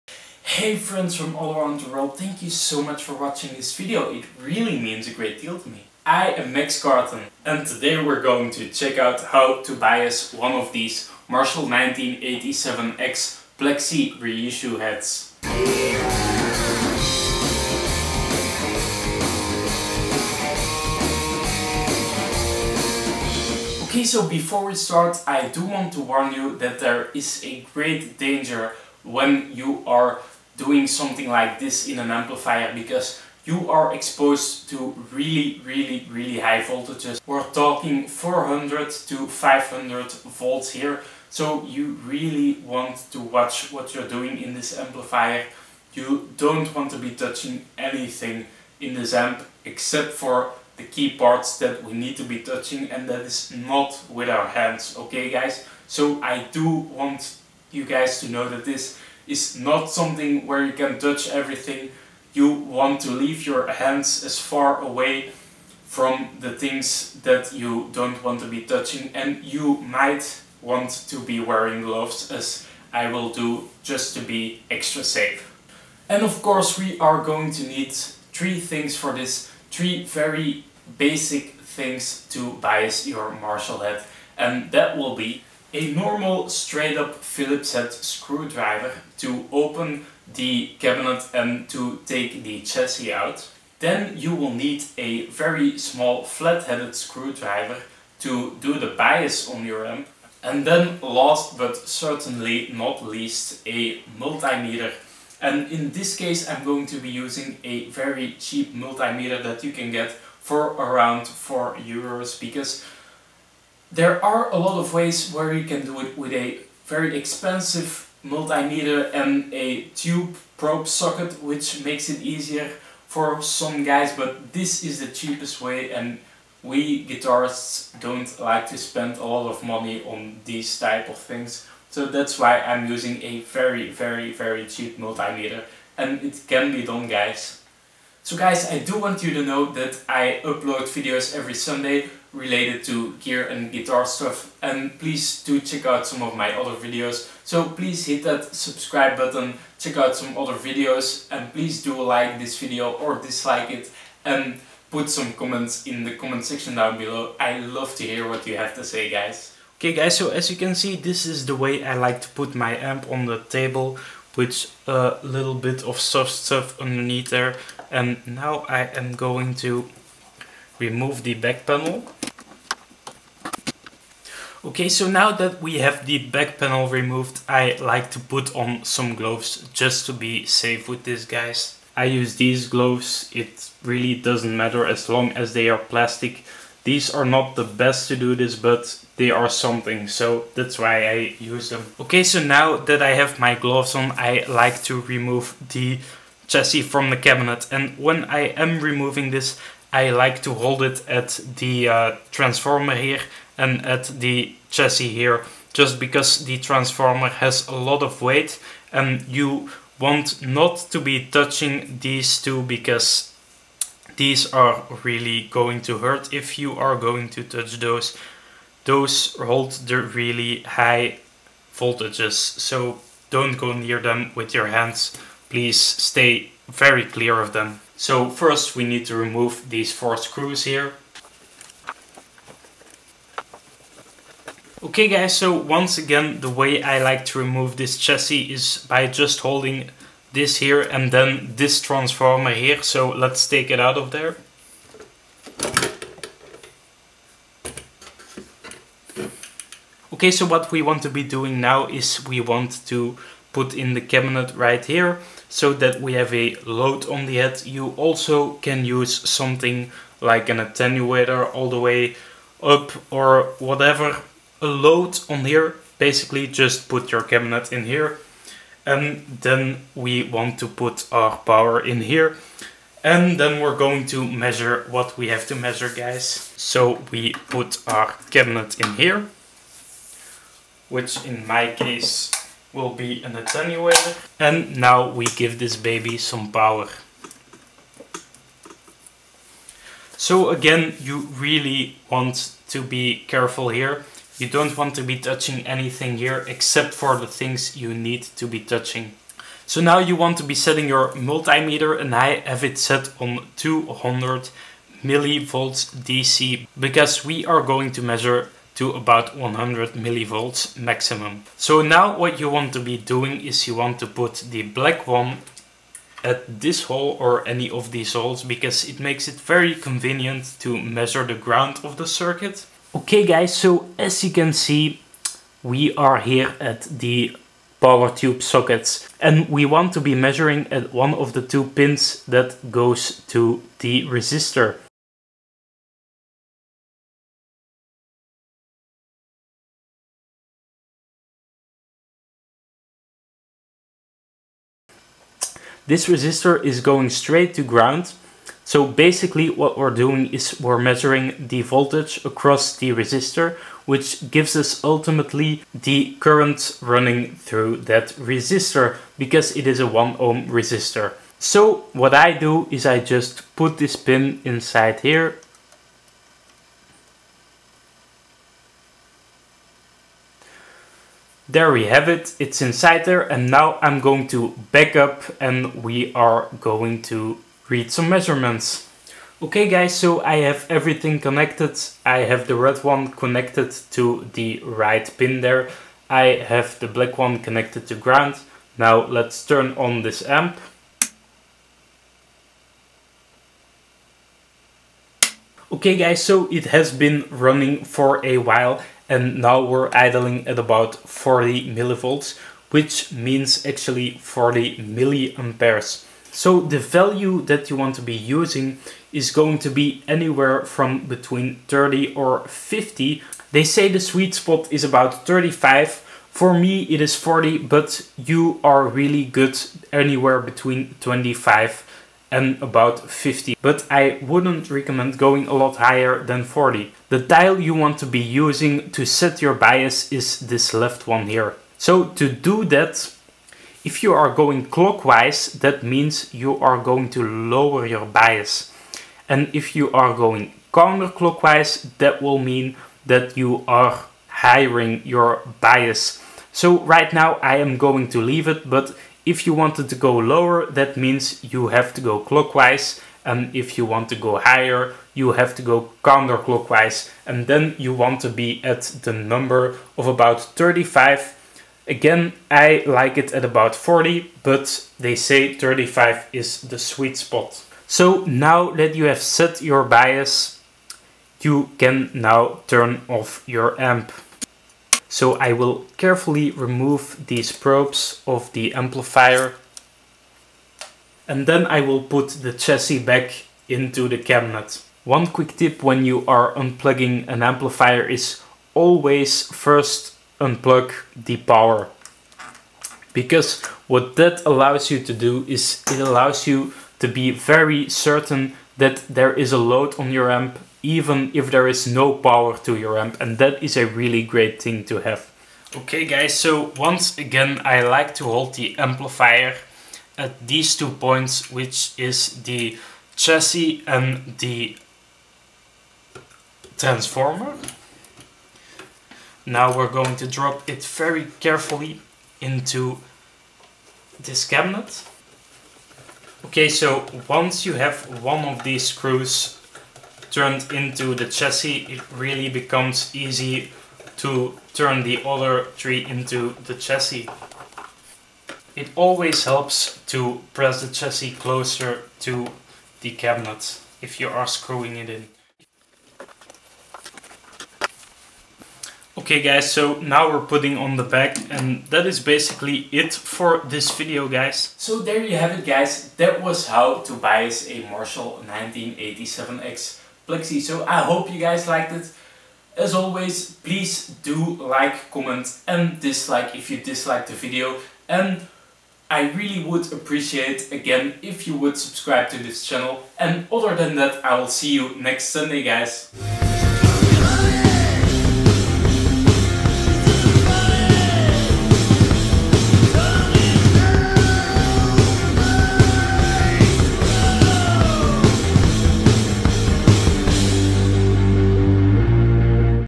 How To Bias: Marshall Plexi 1987x/1959SLP | 50w/100w | Using Multimeter | Valve/Tube Amp
This is a video on how to bias a Marshall Plexi amplifier using only a multimeter. This method should work with any EL34 equipped Vintage series Marshall (1987x, 1959SLP, JTM45, 1959HW,…). If you don’t have a 1 Ohm resistor between your tube sockets and the ground of your amp you either need to solder 1 in for all your sockets. Or you could just buy a ‘Tube Socket Probe’. In that case you’ll need to put your multimeter on the Current measurement setting… In this video I don’t go over how to check the Plate Voltage or how to calculate the right bias setting, since the cheap multimeters don’t measure above 250v. You’ll be fine if you just set your bias to anywhere from 25 to 45, but please stay as low as possible to ensure Tube Life… I use 40mA because it sounds best in my case!